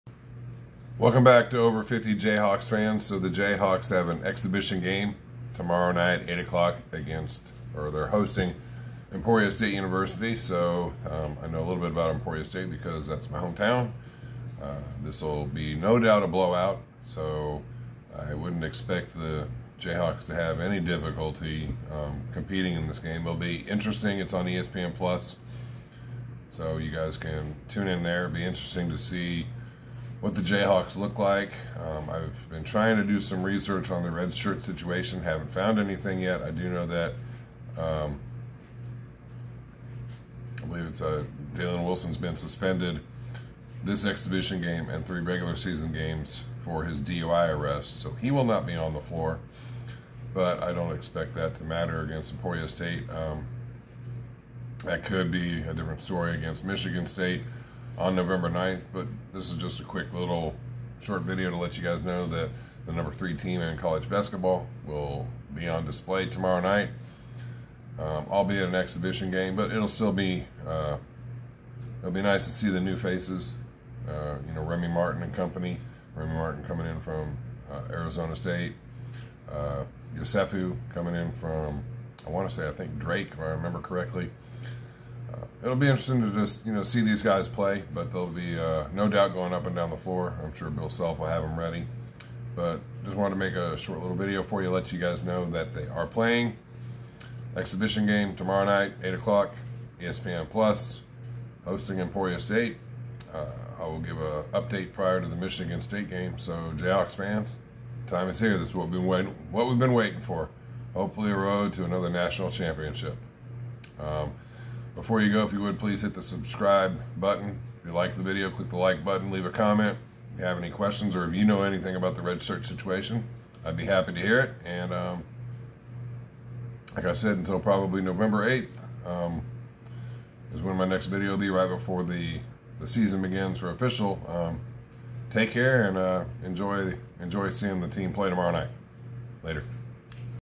Kansas Jayhawks vs. Emporia State Hornets - Exhibition Game 8:00pm Wednesday, November 3, 2021
Perspective and player news ahead of the Jayhawks exhibition game against the Emporia State Hornets Wednesday,  November 3rd at 8:00pm in THE PHOG.